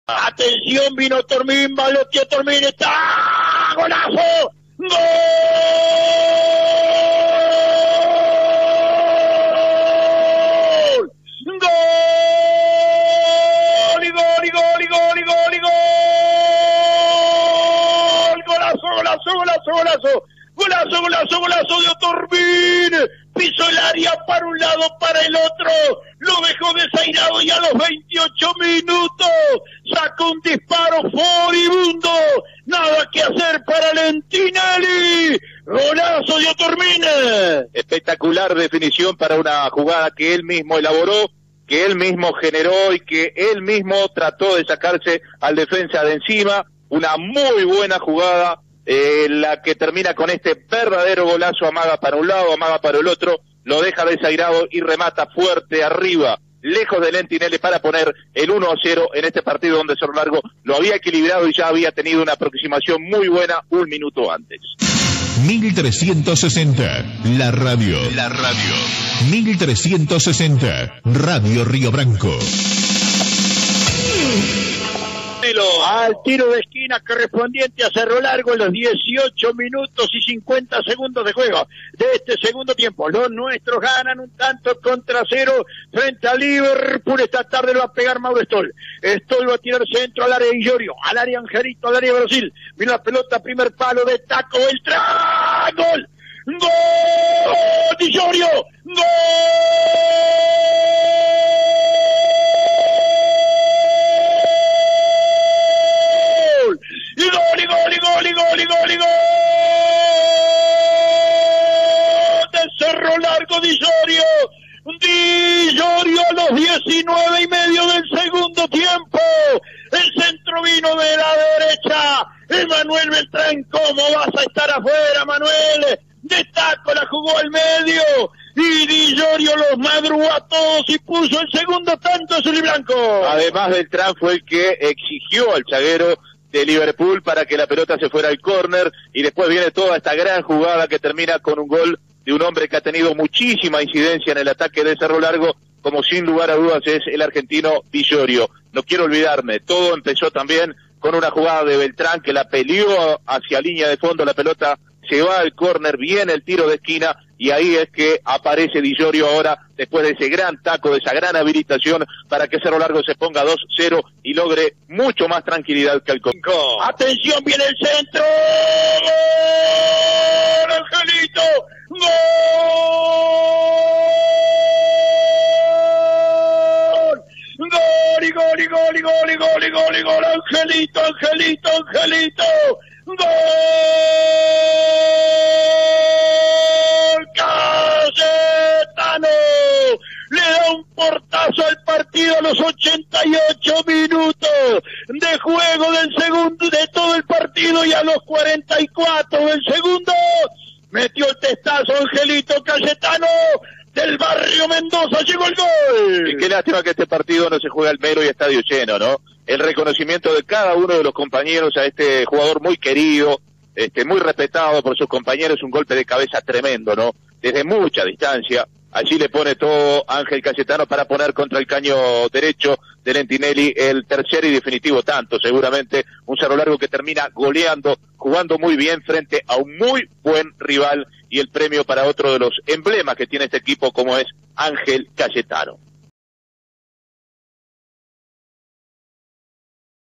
Cerro Largo FC 3 Liverpool 0 - Clausura 2021 (por Roberto Silva)
Los goles del partido, por Roberto Silva y Gerardo Vidales para Radio Río Branco y Acuarela de Melo.